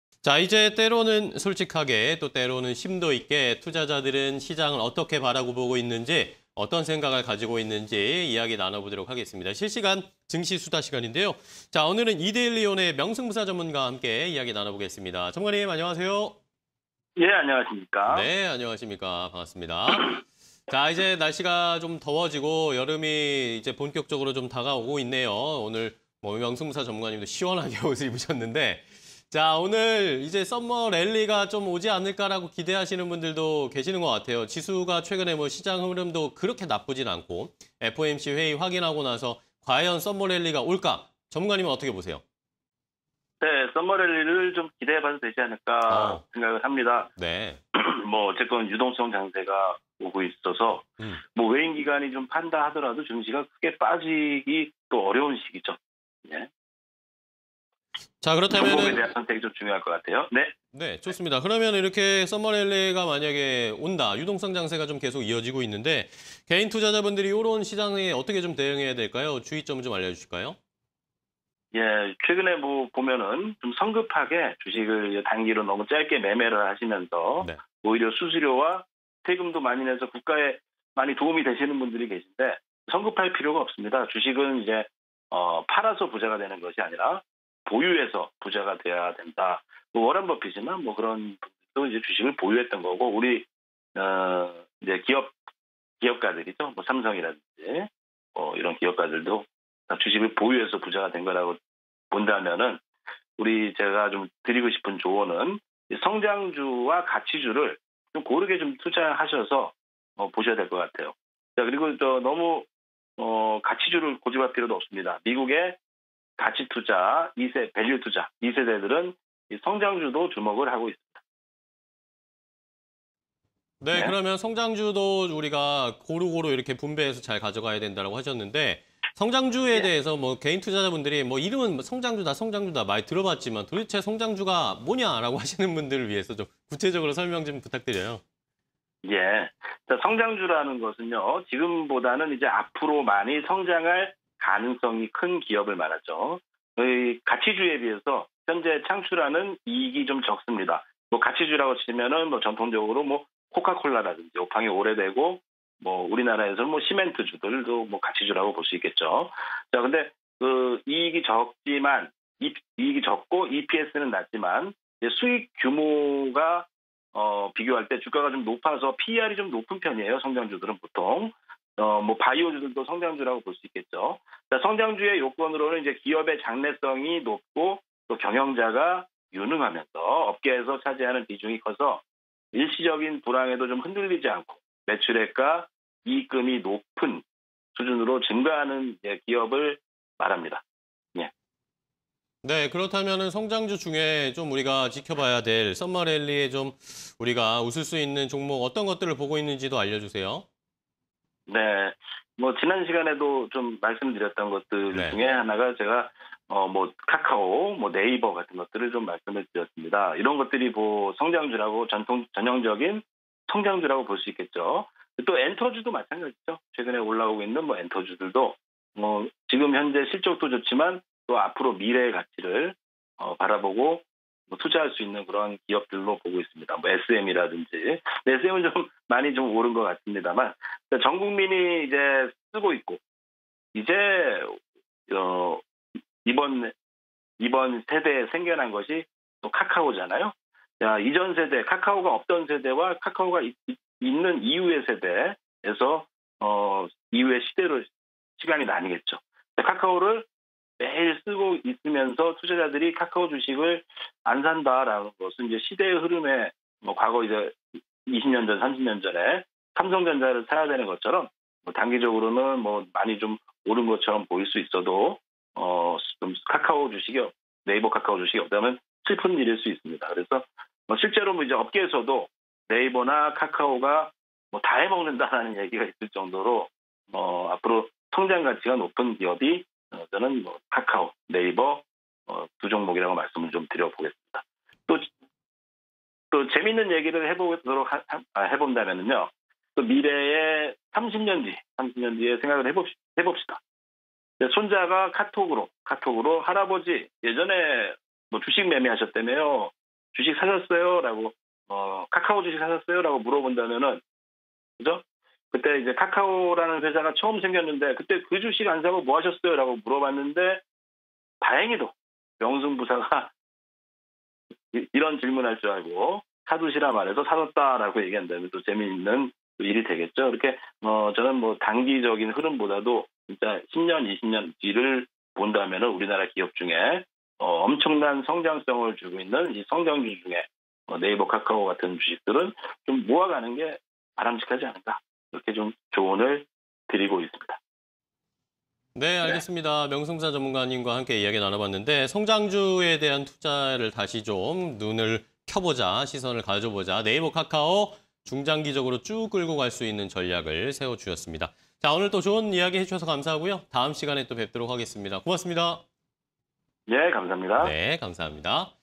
'썸머랠리' 기대감 확대_실시간 증시 수다 (20210615)
Q. 코스피 사상 최고가… '썸머랠리' 오나?
- '썸머랠리' 기대감 확대… '유동성 장세' 예상
- 성장주-가치주, 고른 투자 전략 필요

Q. '성장주' 관심종목은?
- 카카오·네이버, 대표 성장주… 성장성에 투자